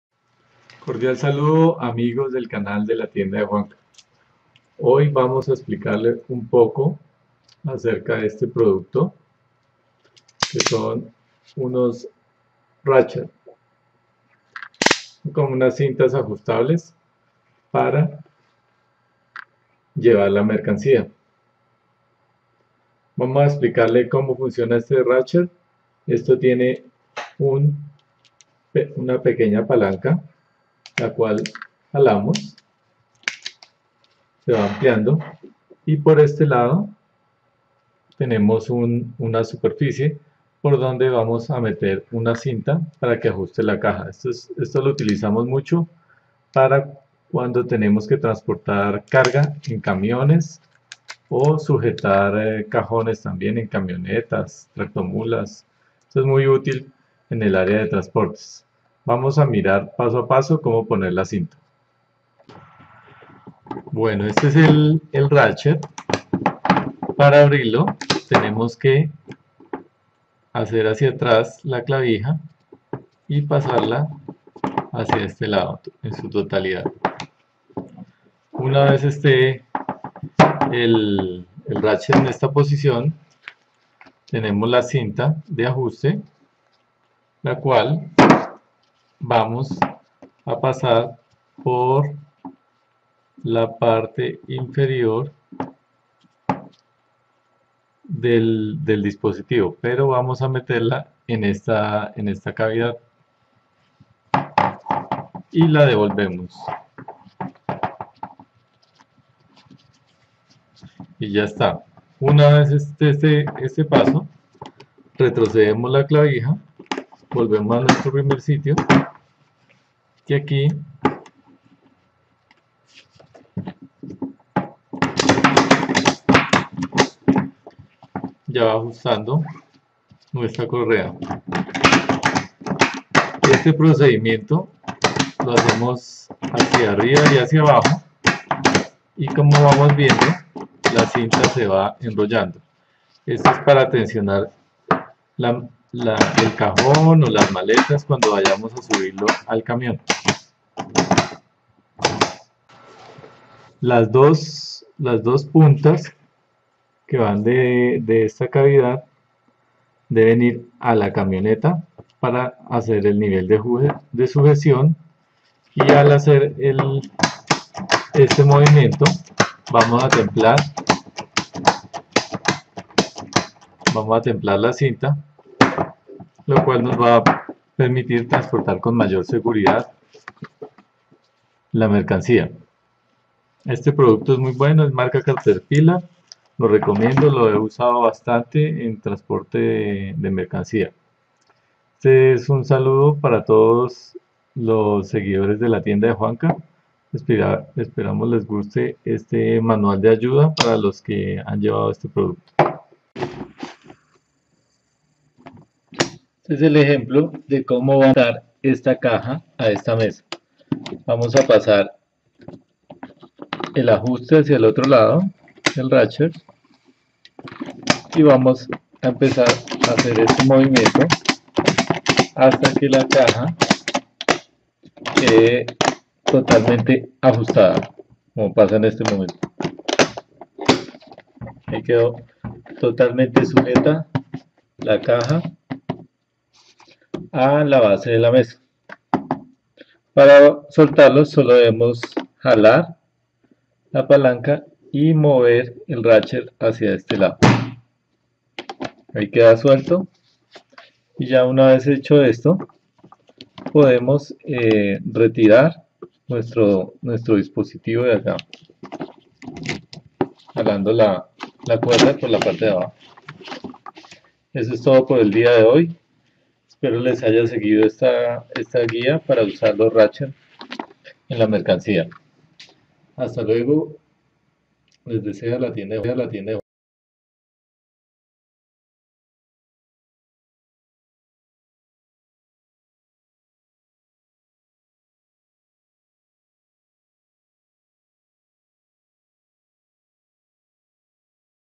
como funciona ratchet caterpilar cinta de seguridad mercancia
como fijar una cinta ratchet o lazo para ajustar la mercancia